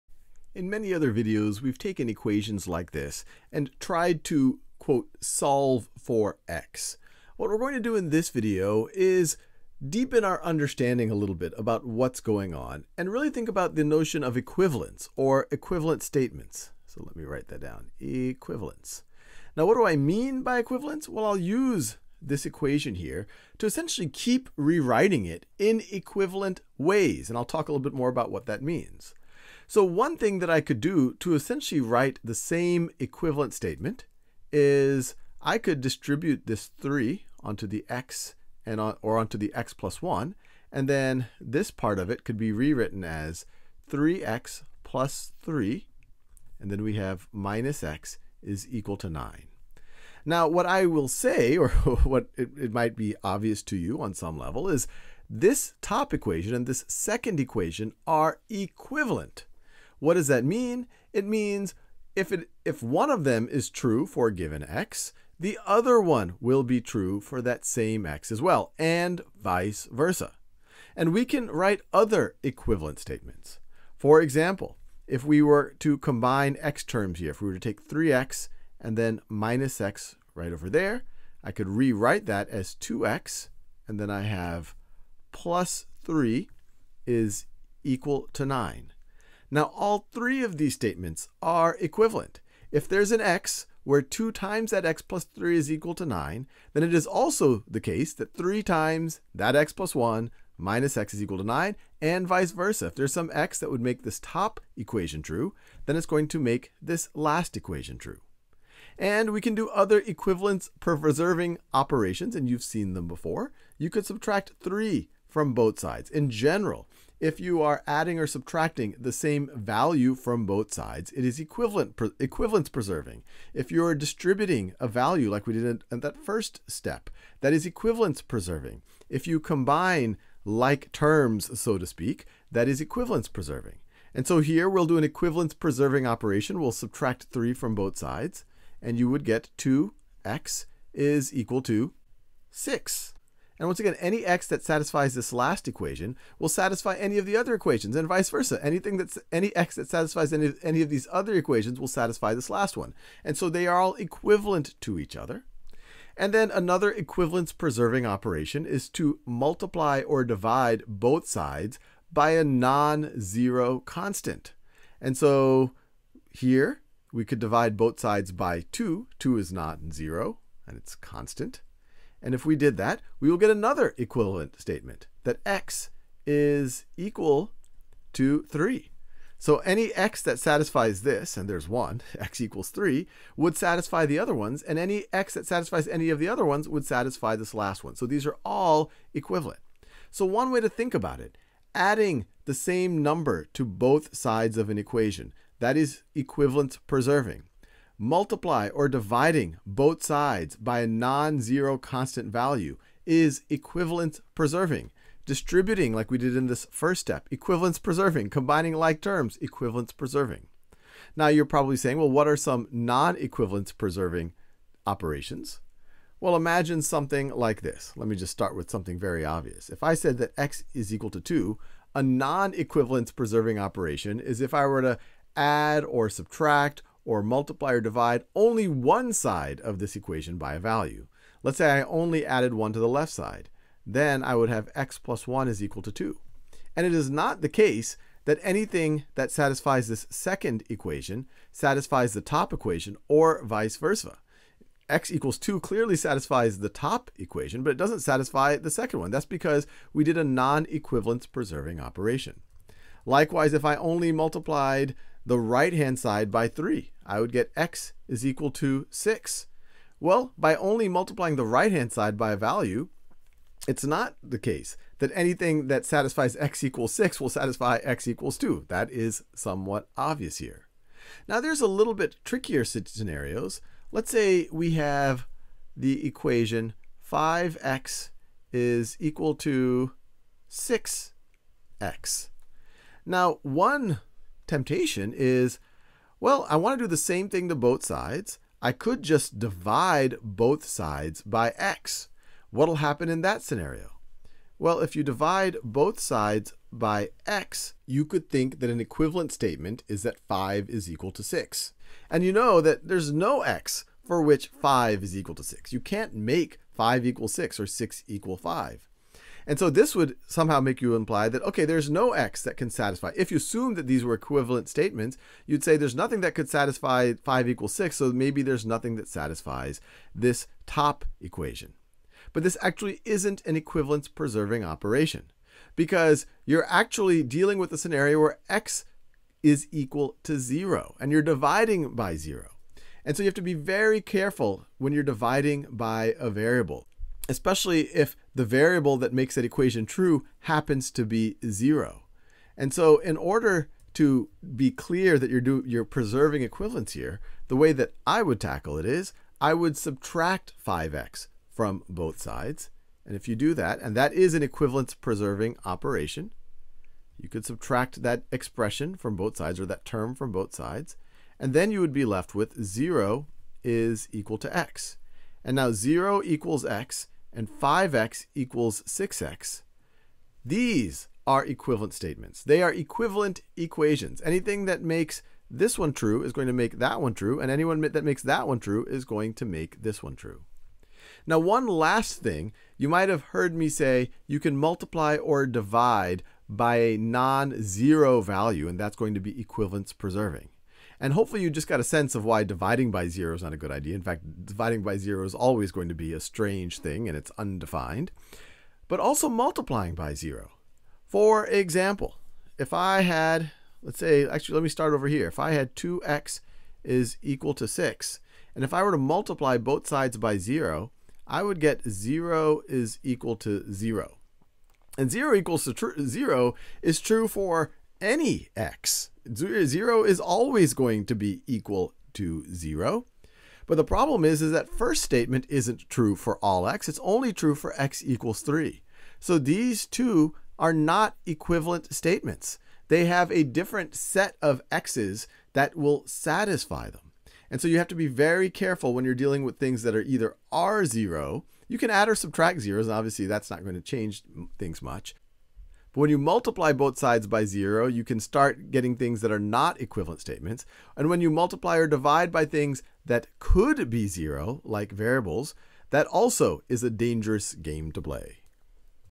Reasoning with linear equations | Solving equations & inequalities | Algebra 1 | Khan Academy
When we perform operations to manipulate equations, some operations produce equivalent equations, while others don't necessarily produce equivalent equations. When we're solving an equation, we need to use operations that guarantee equivalence.